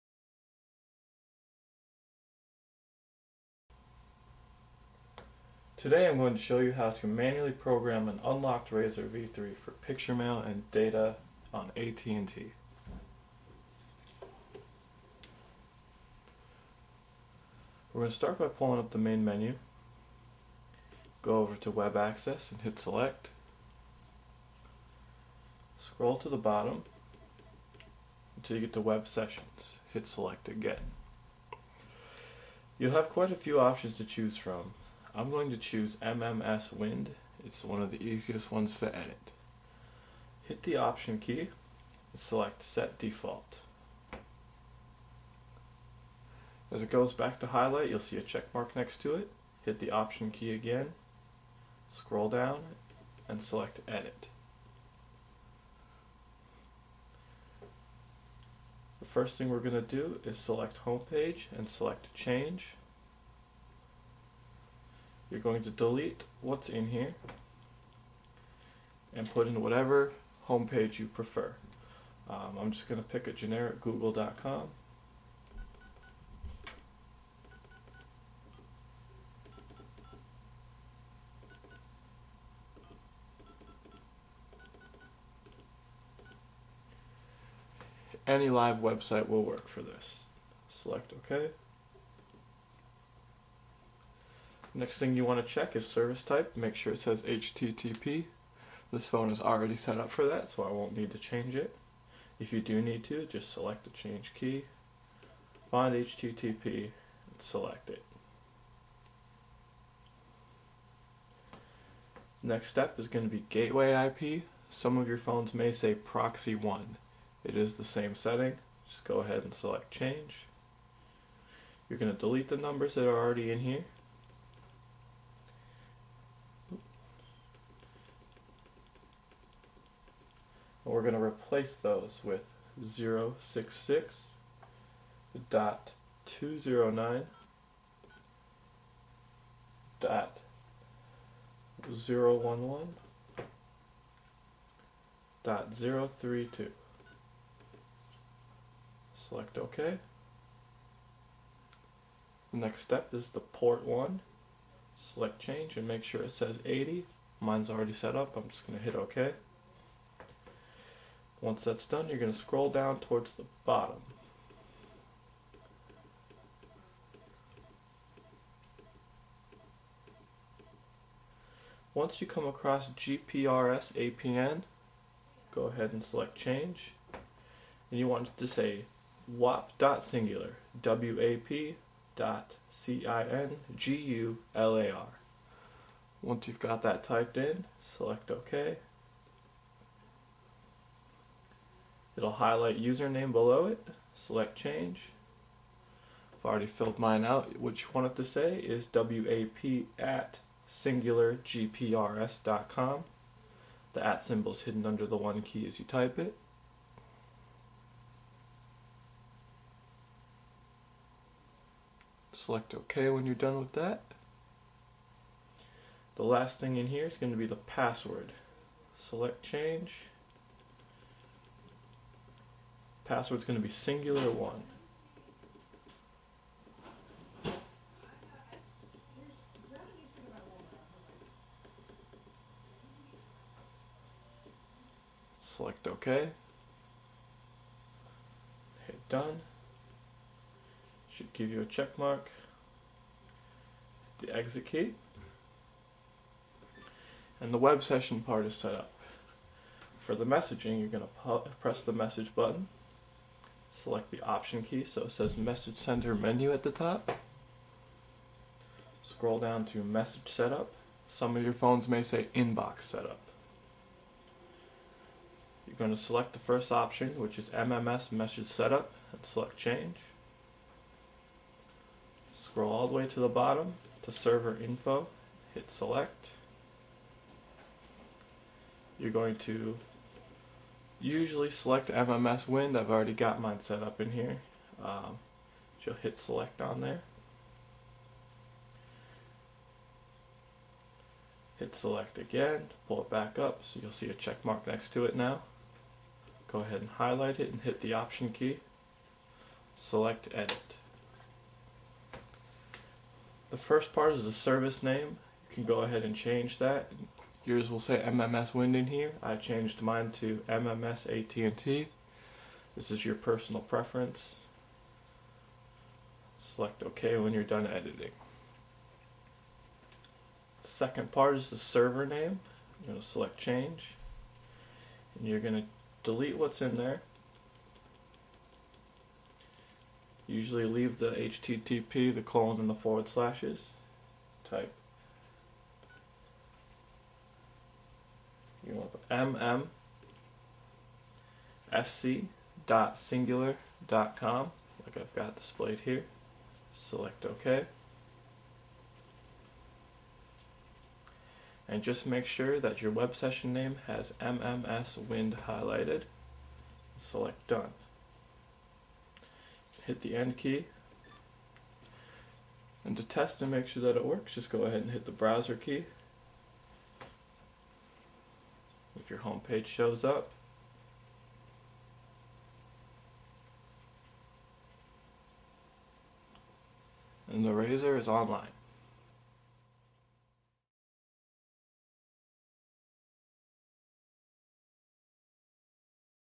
How To Program Unlocked Razr v3 For MMS And Data Settings On AT&T
Tutorial / Instructions on how to program your Unlocked Razr V3 for Picture mail and Data on AT&T.

here are also written directions

At&t MMS setup

messages  (options key)  message setup  MMS Message Setup  Server info

highlight (Newworld or 3db mms) and press the (option Key)  edit.

select and change the following 3 options as shown below.

Service Name -  CINGULAR MMS

Web Session Name - select (3 new World)

Hit done. then go back to your home screen.

Hit the center button (bring up applications). Then Go to web access Scroll Down to

web sessions and hit select.

highlight New world and hit the (option Key).

set as Default.

Hit the (Options Key) again and select Edit.

and hit ok, ... any serch engine will work.

Change service type to - HTTP and hit ok

Change (Gateway IP 1) or (Proxy 1) (depending on what software version you have.) to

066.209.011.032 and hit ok

Change (Port) to 80 and select ok.

scroll toward the bottom until you come across (GPRS APN) and hit change.

change the GPRS APN to - wap.cingular and hit ok

change password to - cingular1  then hit ok.

select done, then hit the red end key to bring you back to youre home page and your ALL DONE